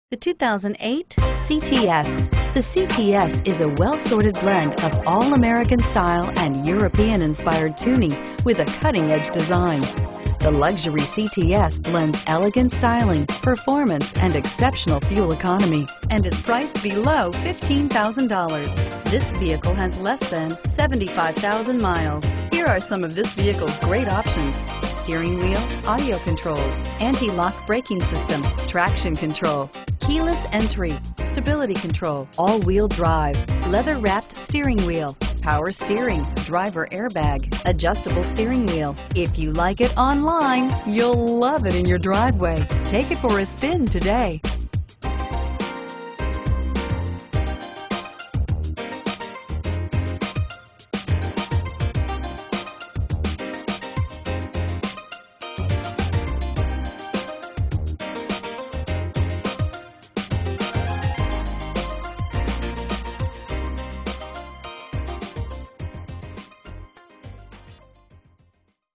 2008 Cadillac CTS Temple TX 129051A
2008 Cadillac CTS AWD w/1SB - $14236

5255 I H 35 South

Looking for the right 4dr car? Today could be your lucky day. With 72657 miles, this BLACK, 2008 Cadillac CTS equipped with Automatic transmission could be yours. Request more information and set up a test drive right away.